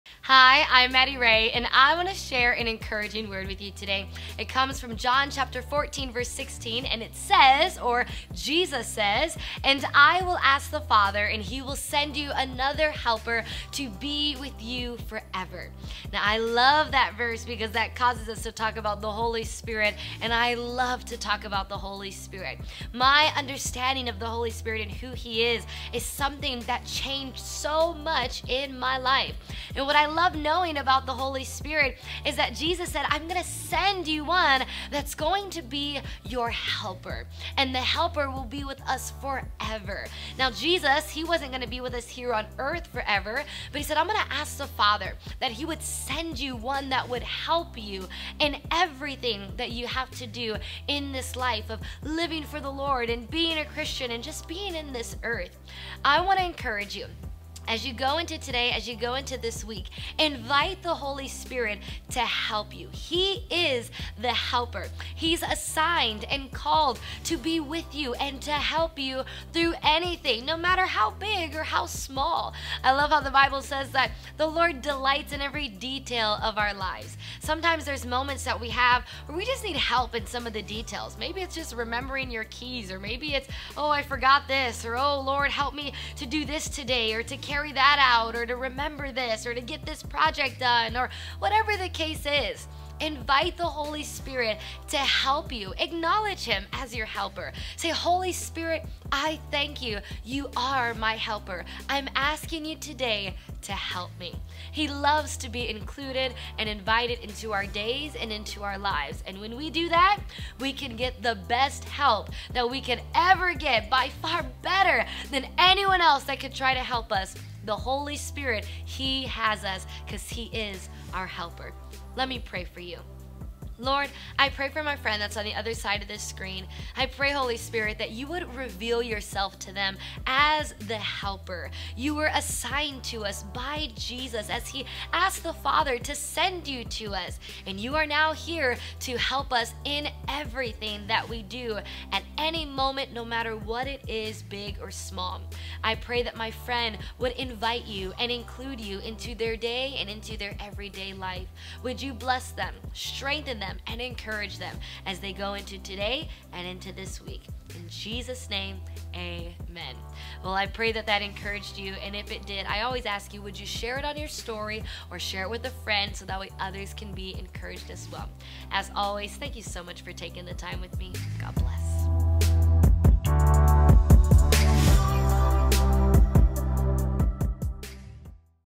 MR Verse of the Week - October 14th, 2024
Verse of the Week - John 14:16

I want to encourage you to go into your week knowing that Jesus has sent us the one that will be our helper which is the Holy Spirit! The Holy Spirit helps us with everything in life. Living for the Lord, being a Christian or just being on this earth. The Holy Spirit is your helper and he is called to be with you at all times. He is assigned to help you with every situation no matter how big or how small. So I encourage you this week to invite the Holy Spirit into your everyday life, so you can receive help in any moment of need!

If this video encouraged you, please share it with a friend or on your story so others can be blessed by it as well!✨